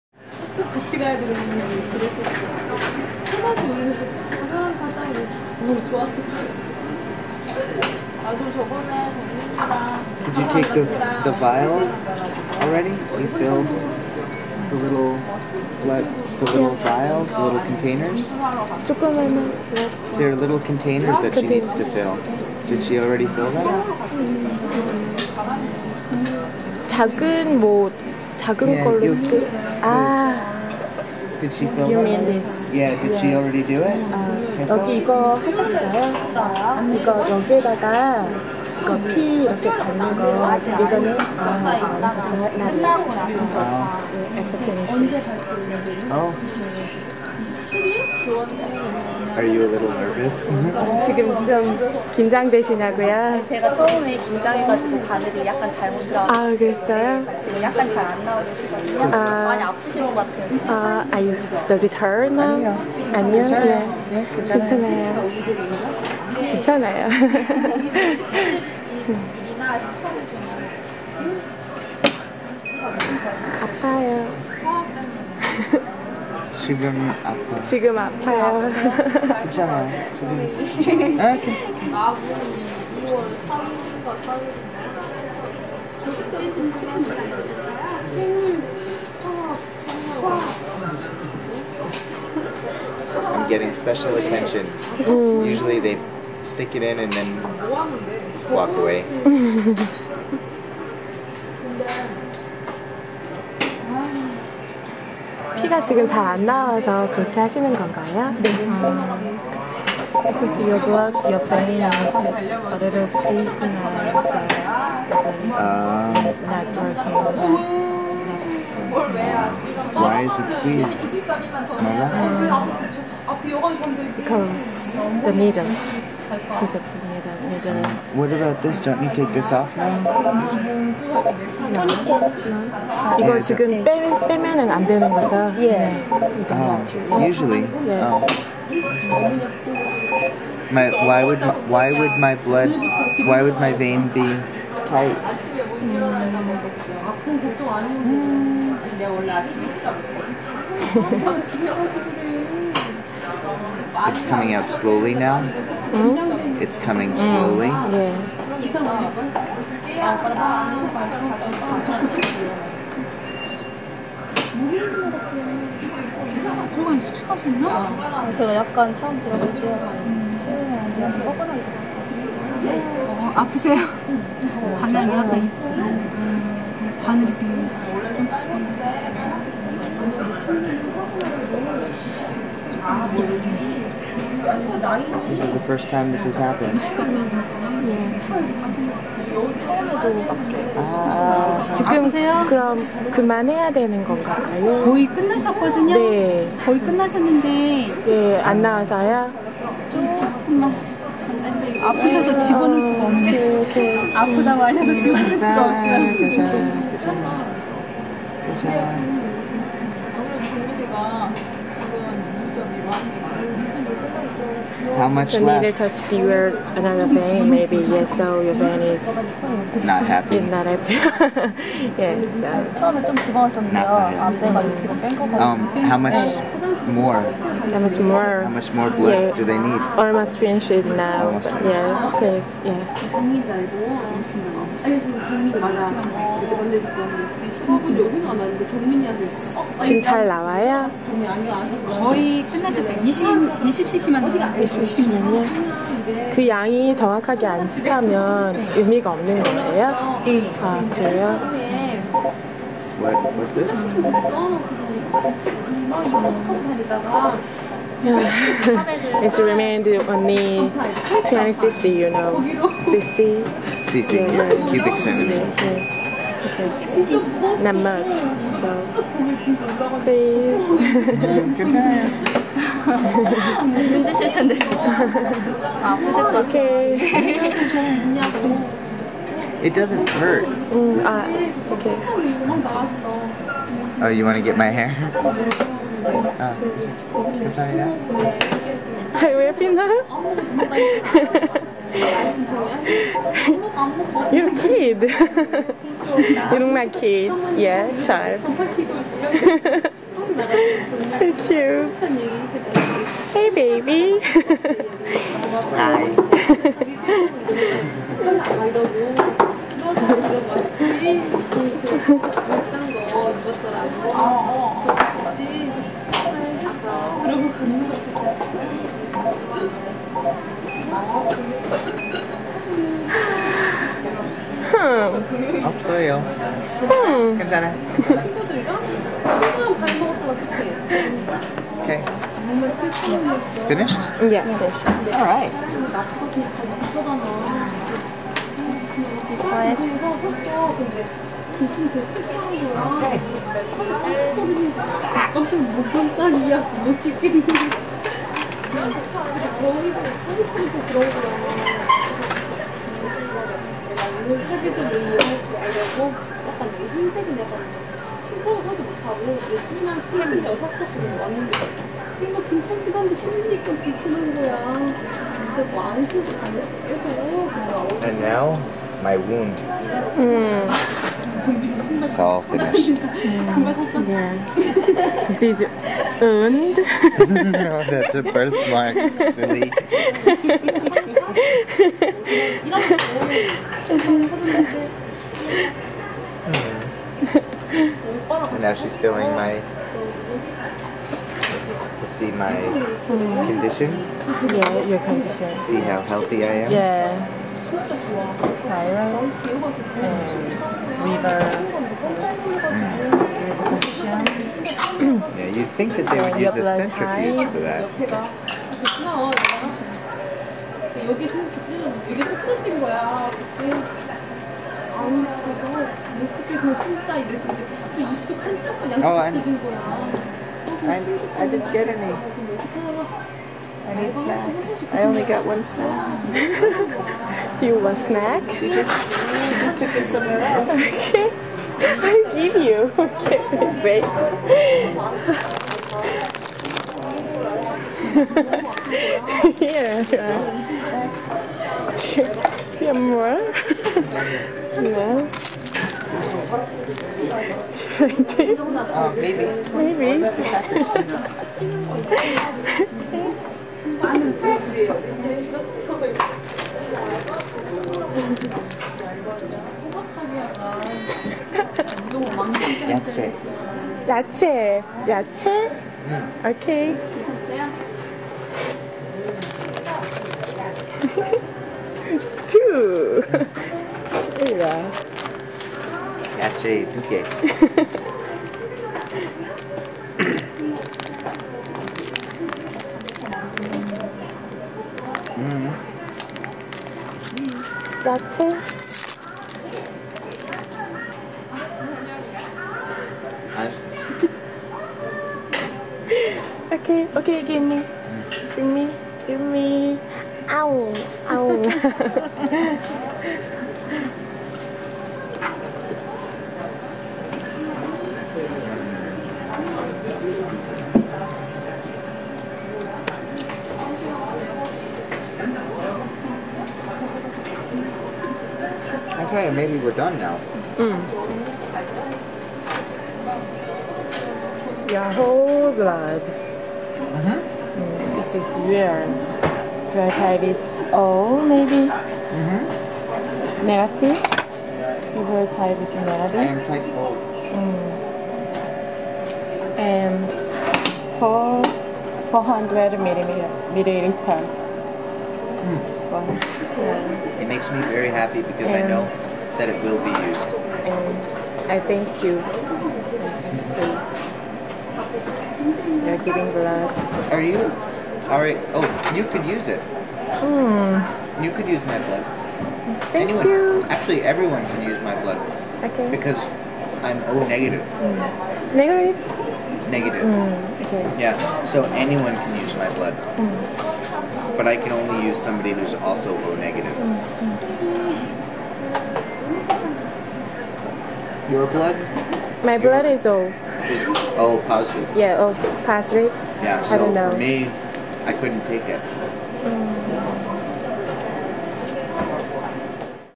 Giving Blood
Here I have proof positive that I gave blood in South Korea with no problem! Really. Again, this probably isn't interesting at all to anyone else, but it makes me smile.